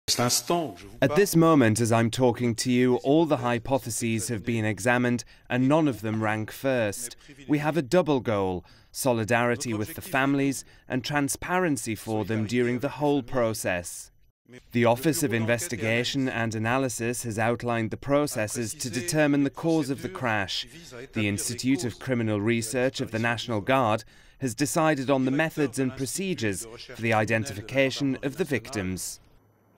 EgyptAir crash: French FM briefs press after talks with families
French Foreign Minister Jean-Marc Ayrault has given a press conference regarding EgyptAir flight MS804, which crashed into the Mediterranean on Thursday. He said France has sent a plane and a naval ship to join the search for wreckage, and vowed to stand with the families of the victims during the investigation and search process.